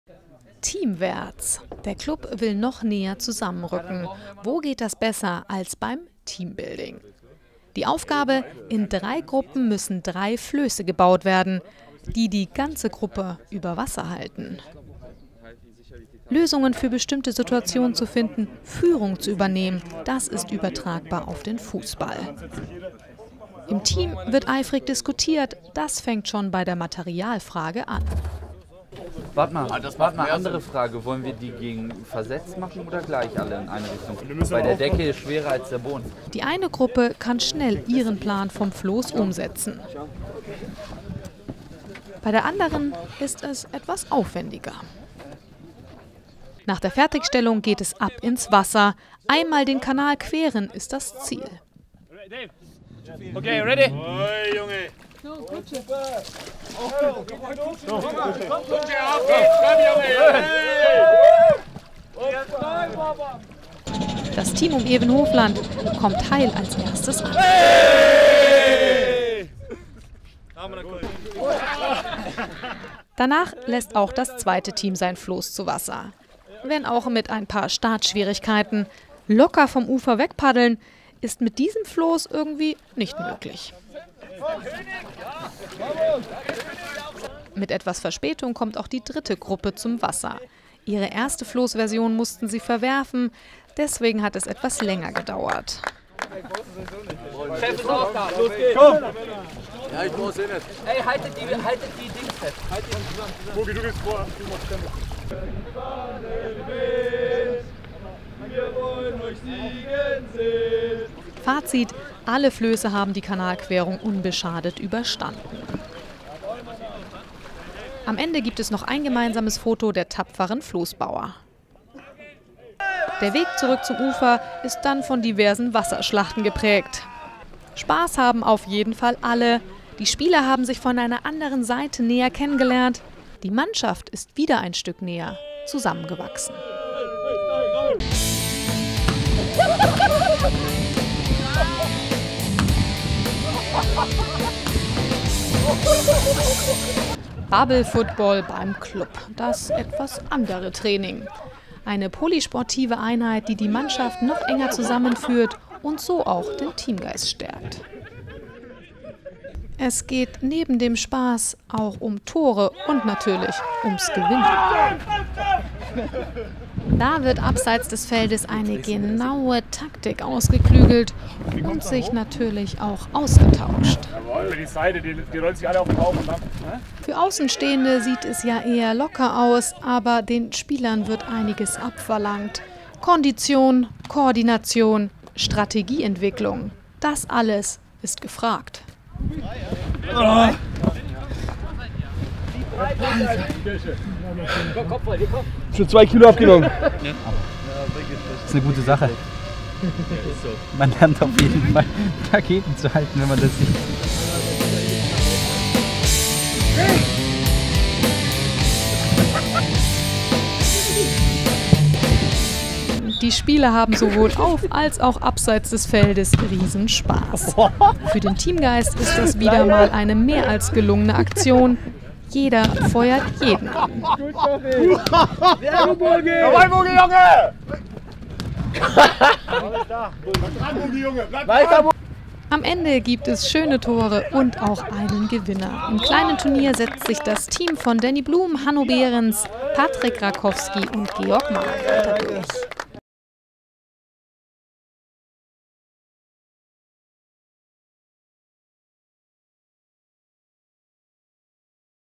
Best of CLUB TV 2015 - Teambuilding Teil 2
CLUB TV zeigt nochmal die besten Szenen abseits des Platzes. In diesem Jahr waren die lustigsten Einheiten die poly-sportiven Einheiten. Neben dem Spaßfaktor spielt hier der Teamgedanke immer mit, die Mannschaft soll näher aneinanderrücken.